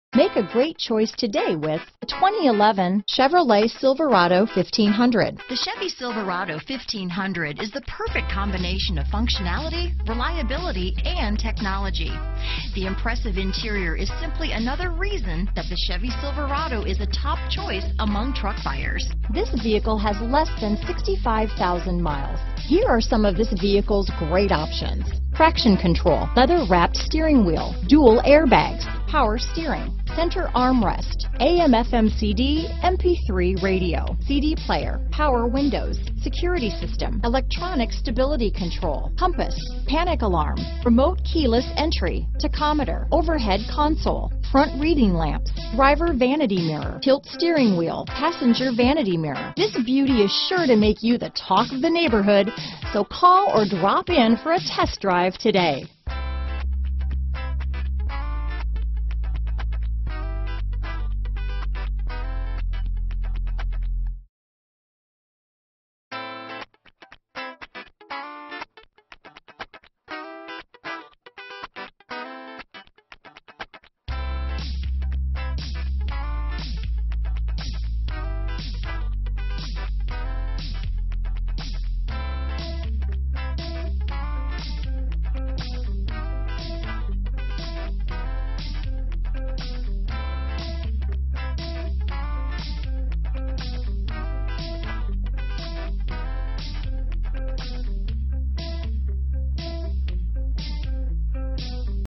2011 Chevrolet Silverado 1500 Morrow, Peachtree City, Newnan, McDonough, Union City, GA W322690
Summit White Used 2011 Chevrolet Silverado 1500 available in Morrow, Georgia at Hennessy Mazda Buick GMC. Servicing the Peachtree City, Newnan, McDonough, Union City, GA area.
2011 Chevrolet Silverado 1500 LT - Stock#: W322690 - VIN#: 3GCPKSE37BG322690